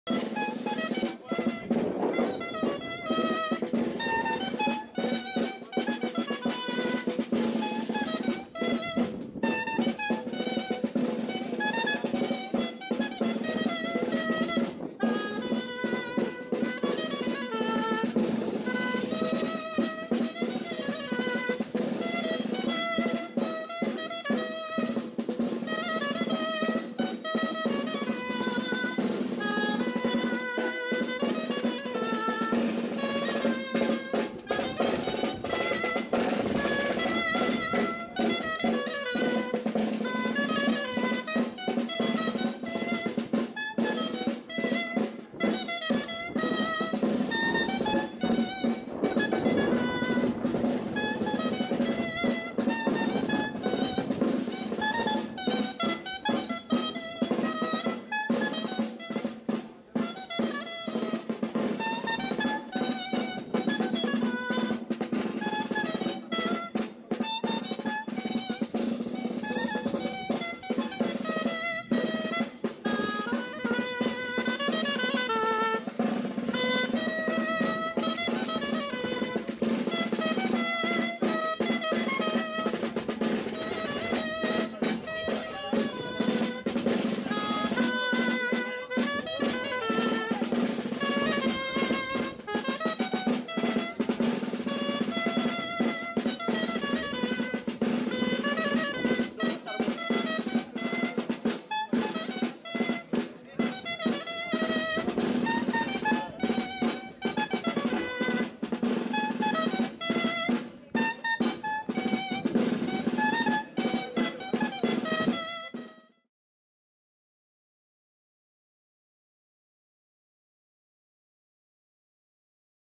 Cieza. Cantabria. Mayo de 2020.
El municipio de Cieza, en Cantabria. Tiene tres localidades, Villayuso, Villasuso y Collado. En este video corto se muestran algunas imágenes aéreas, tomadas en mayo de 2020.